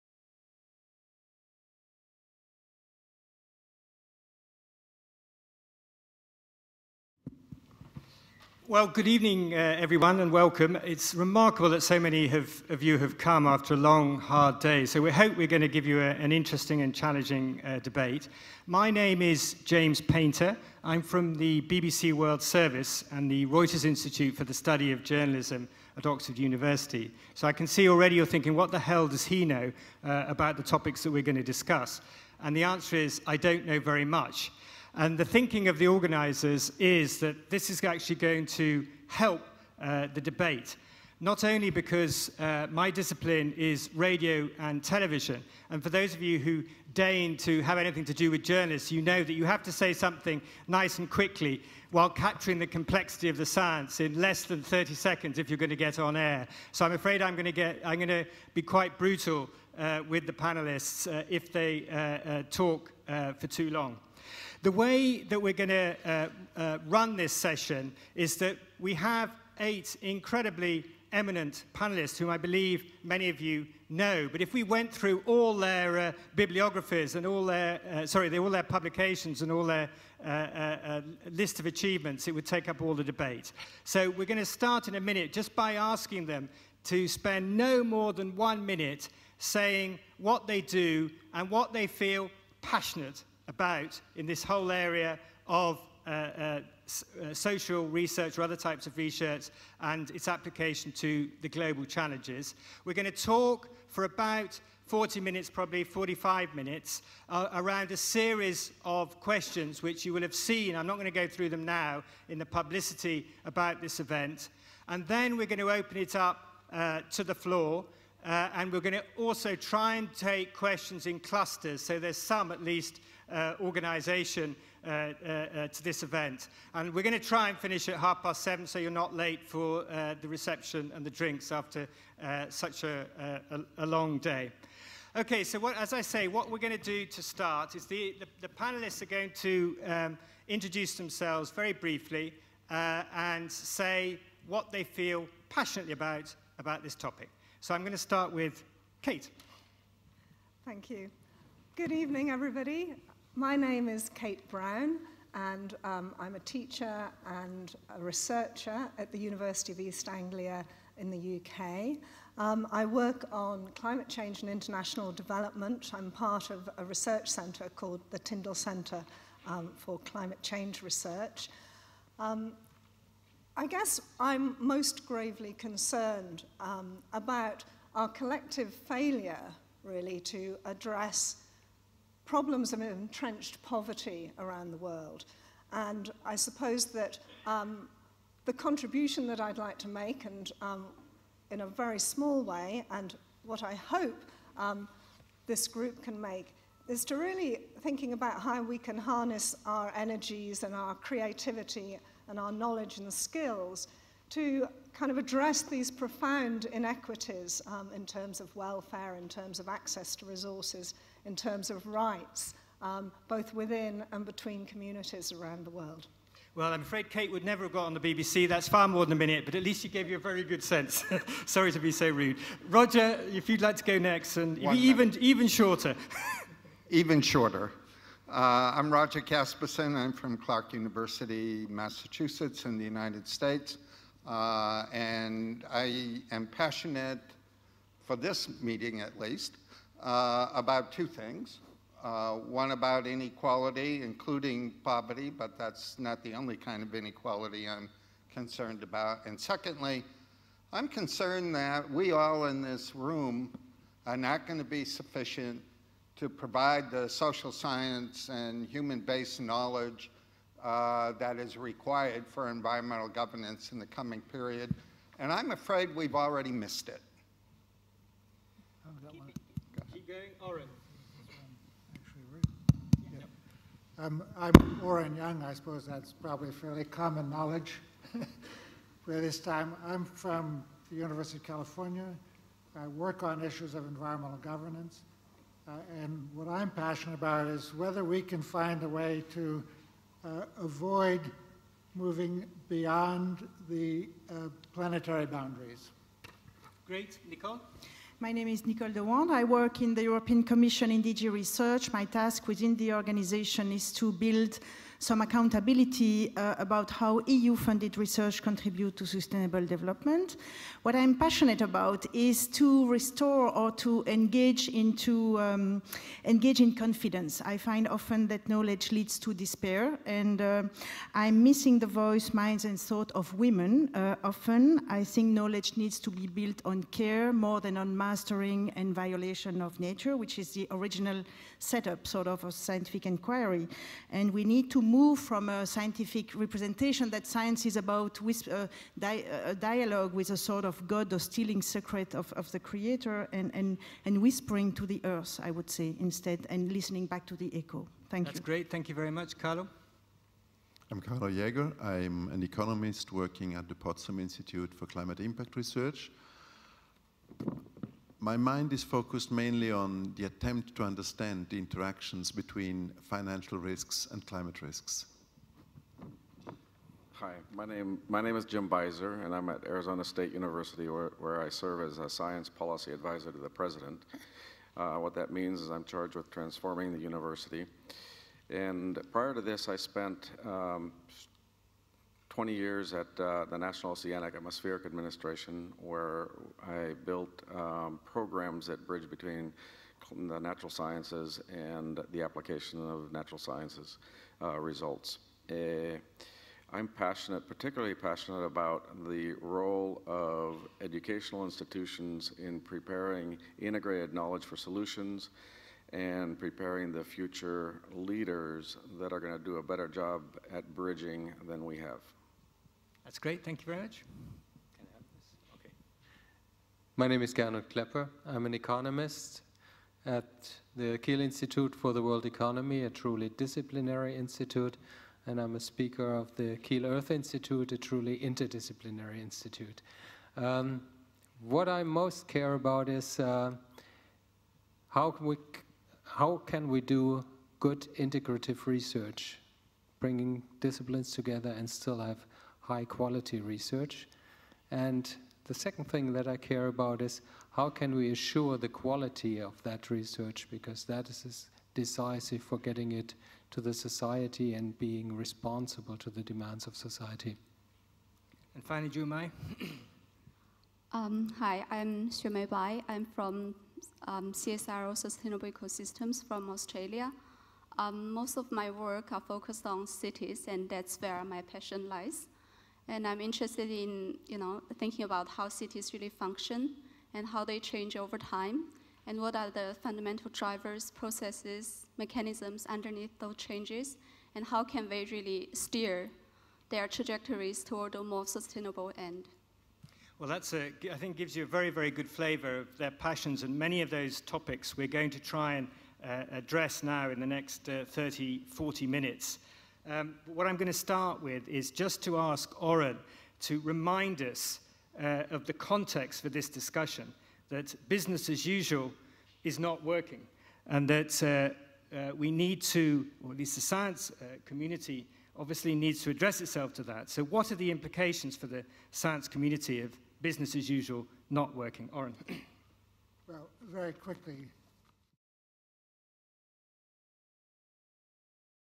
1 of 10 Roundtable - Science of the 21st Century - IHDP Open Meeting 2009
International Human Dimensions Programme on Global Environmental Change (IHDP).

The roundtable, "Addressing the Social Challenges of Global Change - the Role of Science in the 21st Century", will provide a platform for representatives of the human dimensions community and its partners to exchange views on the role of the science in effectively meeting the challenges of global change in the current century.

The IHDP Open Meeting 2009, presented a good occasion at which to discuss such pertinent questions in a public debate.